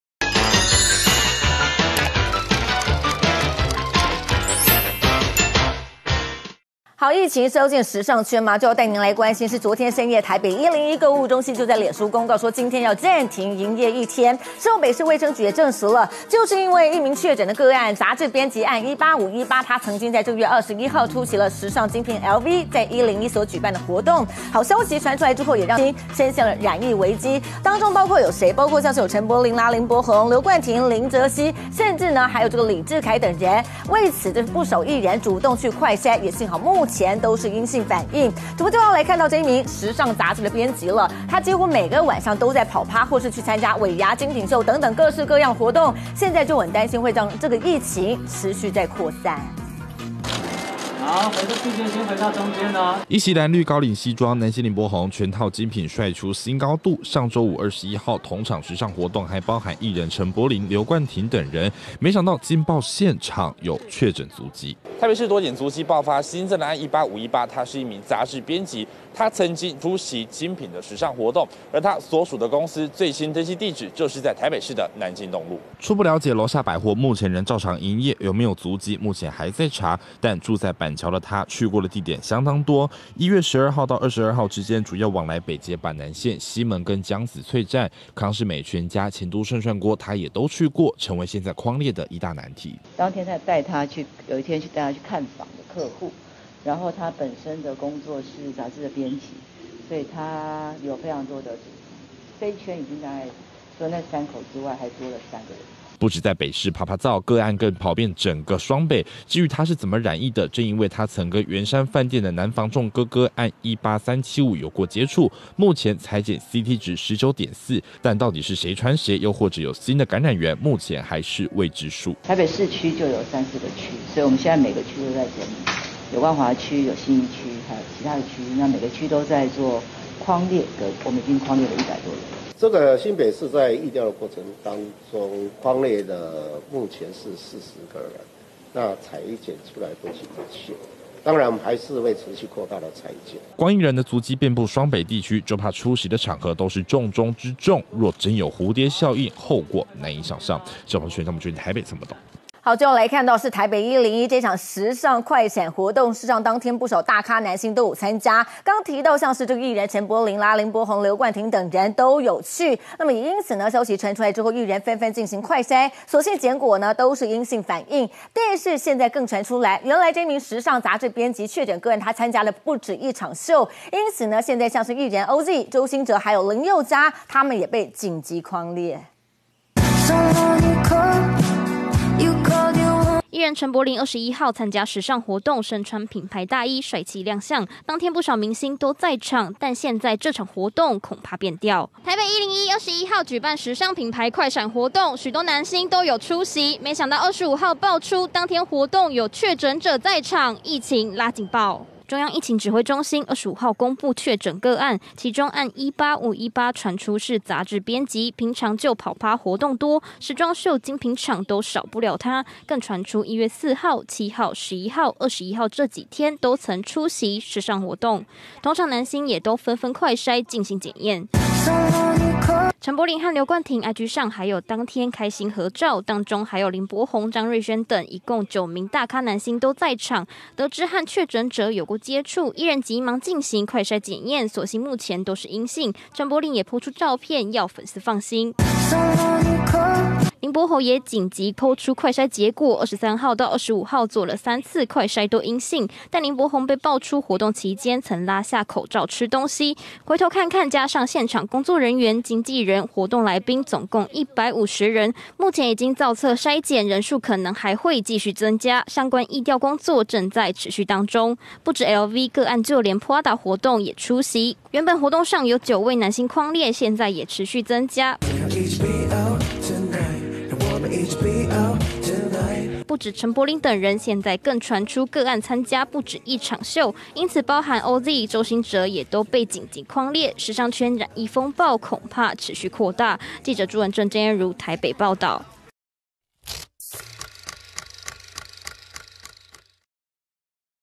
【新聞一點靈】品牌男裝趴有確診足跡！ 北市匡百人 演藝圈掀危機！ 9男星同場急快篩陰性
台灣演藝圈掀確診危機！某位確診的雜誌編輯1月21日出席時尚品牌舉辦的活動，同場男星多達超過9位，目前已在第一時間被通知進行採檢。至於該名確診編輯，主要活動範圍都在捷運板南線，也因為足跡相當廣，目前已經匡列超過百人。

台北101購物中心在1月21日舉辦時尚快閃活動，不少大咖男星都有參加，其中有陳柏霖、林柏宏、劉冠廷等人，但1月25日指揮中心證實案18518的雜誌編輯當天也在場，藝人紛紛進行快篩，所幸結果都陰性，不過現在更傳出，個案參加不只1場秀，因此包含OZI跟周興哲還有林宥嘉，也都被緊急匡列。